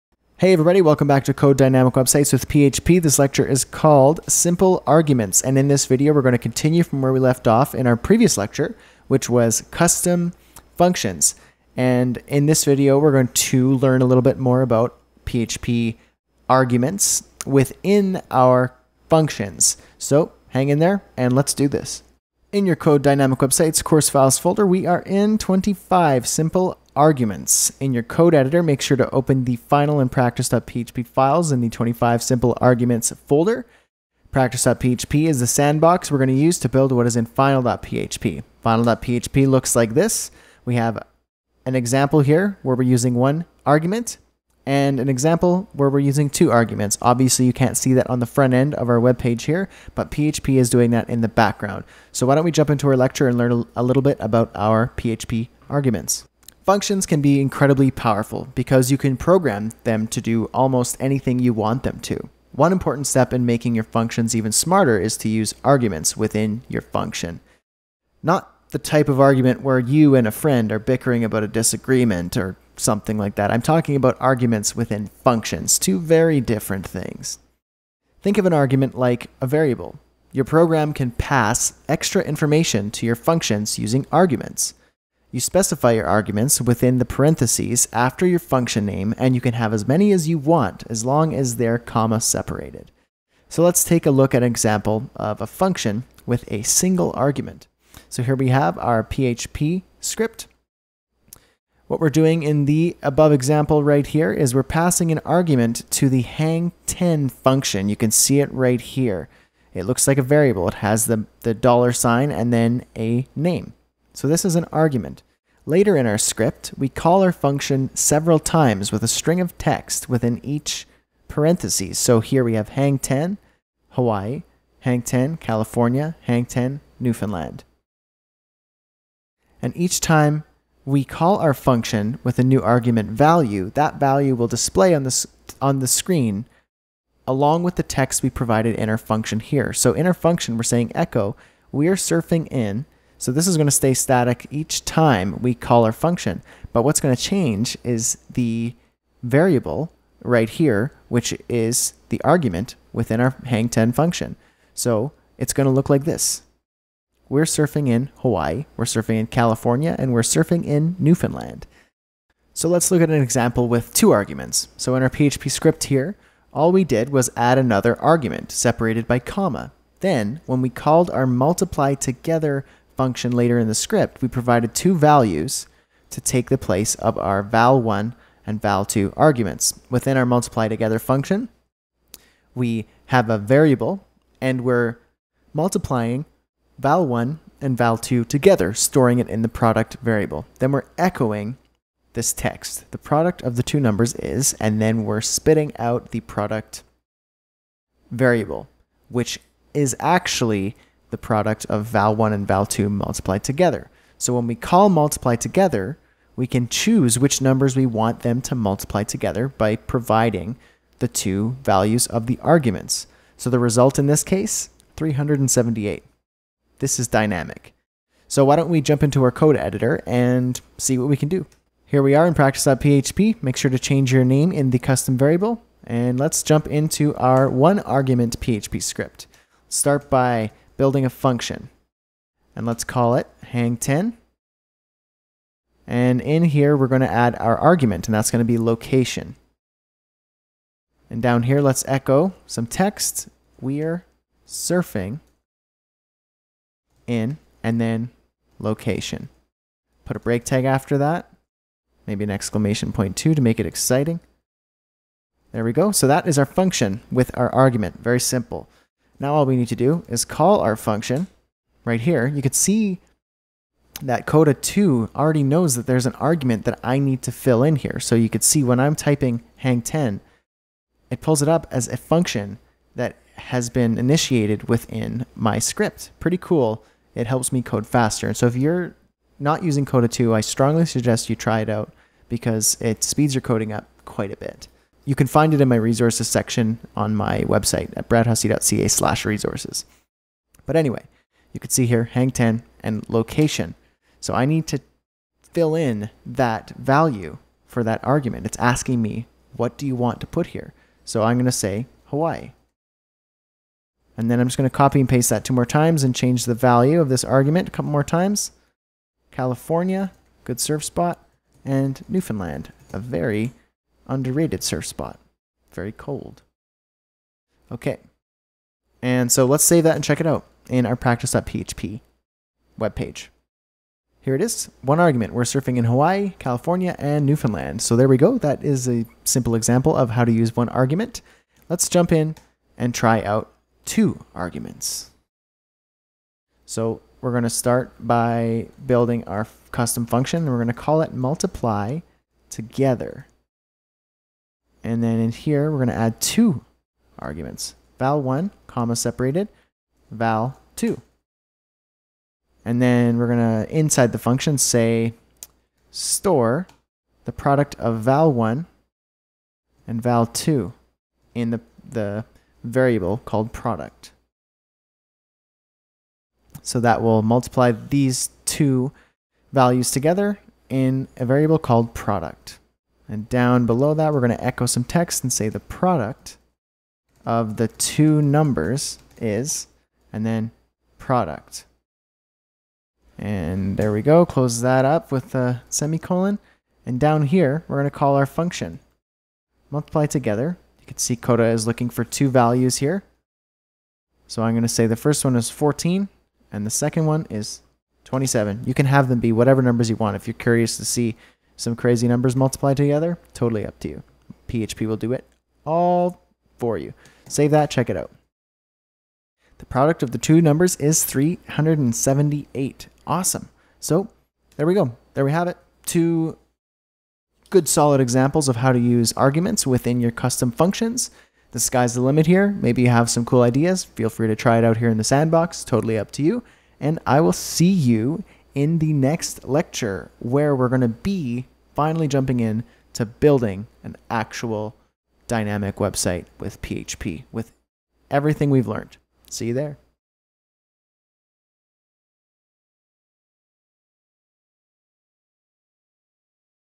Simple Arguments [#31] Code Dynamic Websites with PHP
Lecture 31: Simple Arguments

Functions can be incredibly powerful, because you can program them to do almost anything you want them to. One important step in making your functions even smarter is to use "arguments" within your function. Not the type of argument where you and a friend are bickering about a disagreement, I'm talking about arguments within functions — two very different things.

Think of an argument like a variable. Your program can pass extra information to your functions using arguments. You specify your arguments within the parenthesis after your function name, and you can have as many as you want, as long as they're comma separated.

Let's look at an example of a function with a single argument:

function hangTen($location) {

echo "We're surfing in $location!";

hangTen("Hawaii");

hangTen("California");

hangTen("Newfoundland");

So, in the above example, we're passing an argument to the hangTen() function. Later in our script, we call our function several times with a string of text within each parenthesis, and each time we call our function with a new argument value, that value will display on the screen along with the text we provided in our function.

The output will look like this:

We're surfing in Hawaii!
We're surfing in California!
We're surfing in Newfoundland!

Let's look at an example with two arguments:

function multiplyTogether($val1, $val2) {

$product = $val1 * $val2;

echo "The product of the two numbers is: $product";

multiplyTogether(14, 27);

All we did here was add another argument, separated by comma, then when we called our multiplyTogether() function later in the script, we provided two values to take the place of our $val1 and $val2 arguments.

The result?

The product of the two numbers is: 378